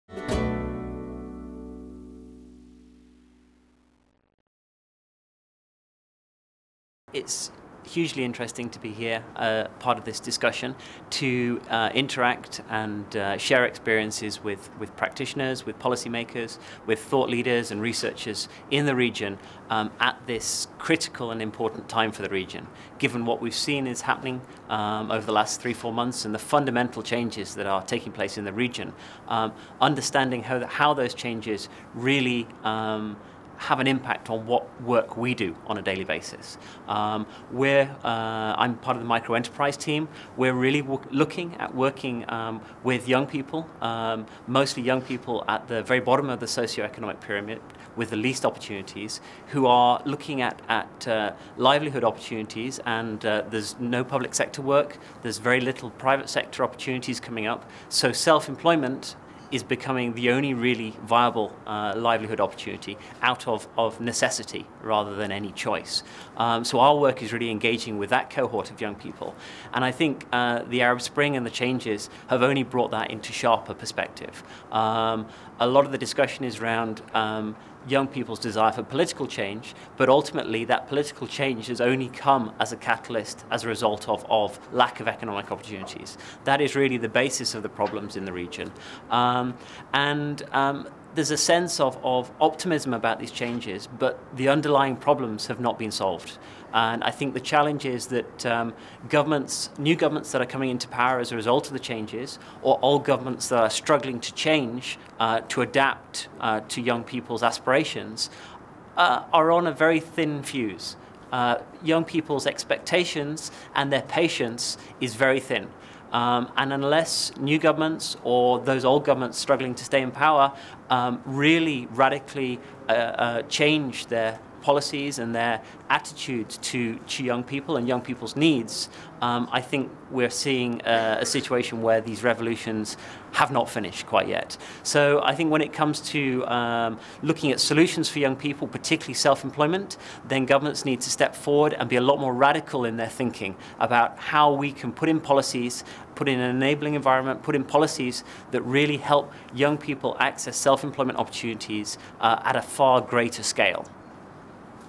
Justin Sykes
Silatech: The Arab Spring Forum July 2011 - Interview with Justin Sykes